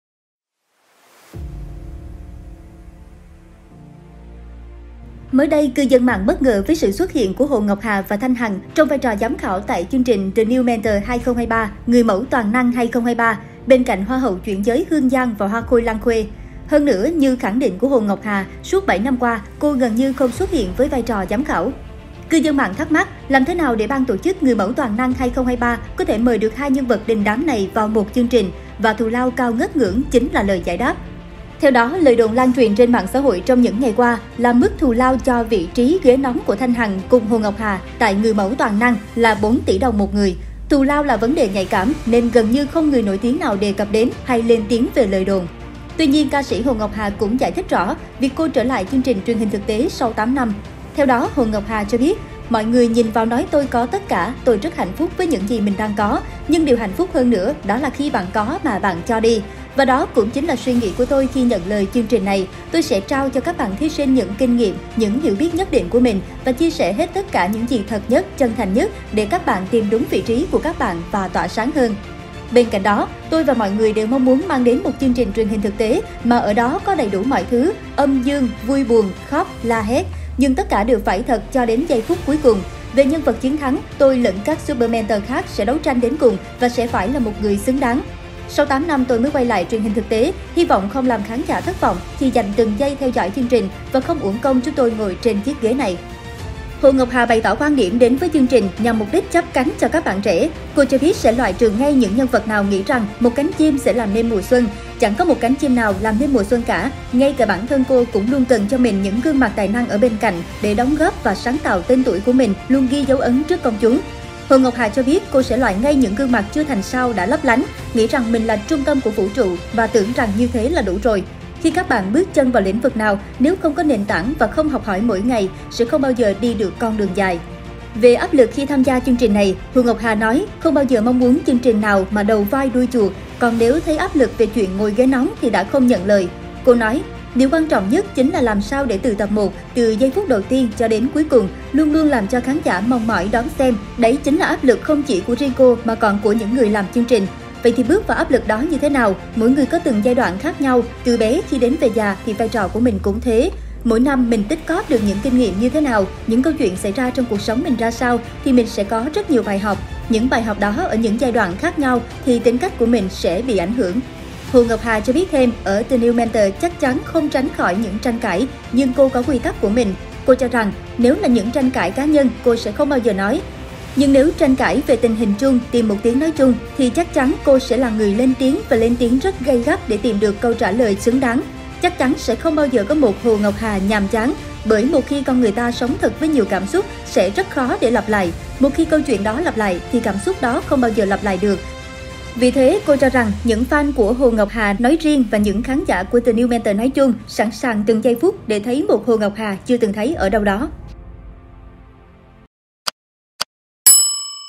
Dân mạng đồn Hồ Ngọc Hà được trả 4 tỉ để ngồi ghế nóng, chính chủ nói gì? | Báo Người Lao Động
Việc ca sĩ Hồ Ngọc Hà xuất hiện tại một chương trình truyền hình thực tế sau 8 năm không nhận lời bất cứ chương trình nào, trong vai trò giám khảo, thu hút sự chú ý.

Link bài viết trên Báo Người Lao Ðộng - MỜI ĐỌC

* Thực hiện: Ngọc Trinh - Ngọc Lý